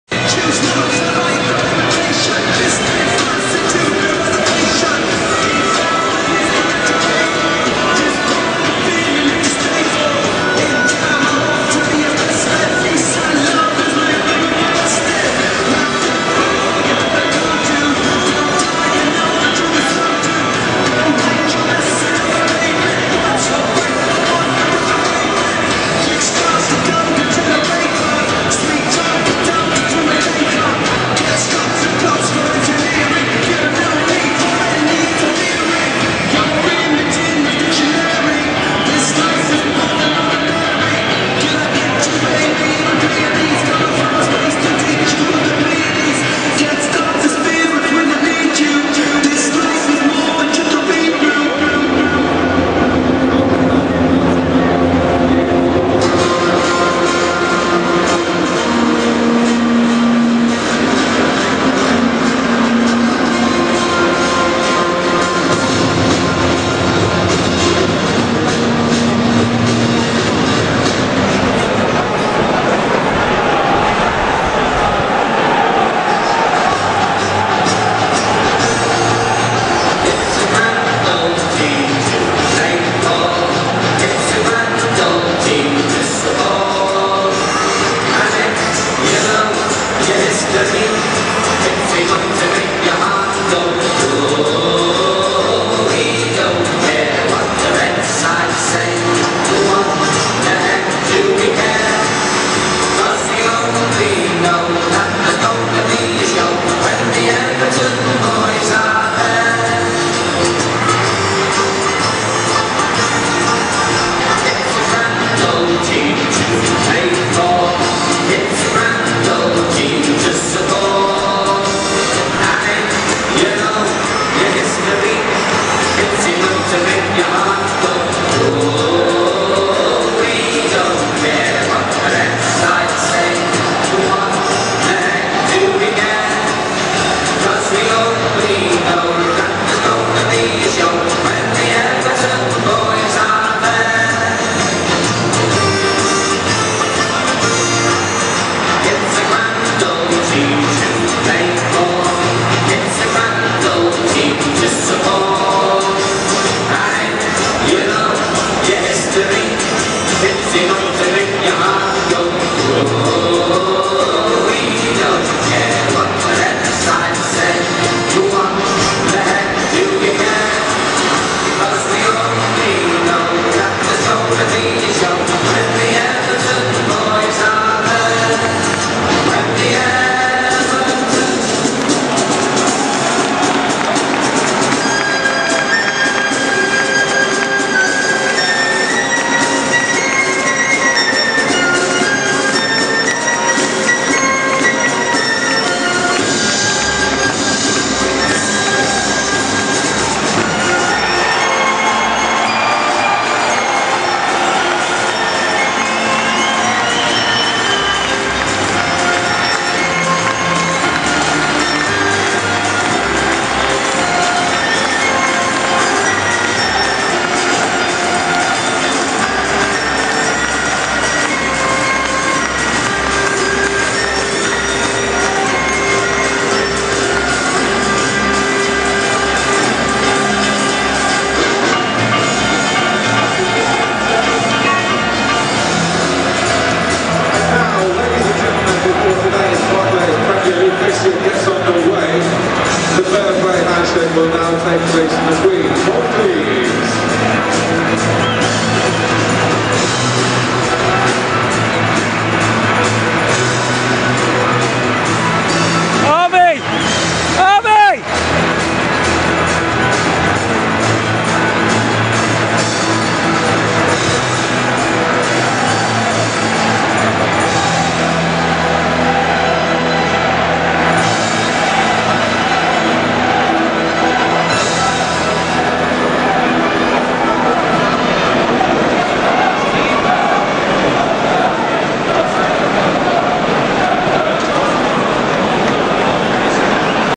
Harvey mascot day
Harvey walks onto the pitch with Tim Howard before the start of the Everton v Norwich game December 2011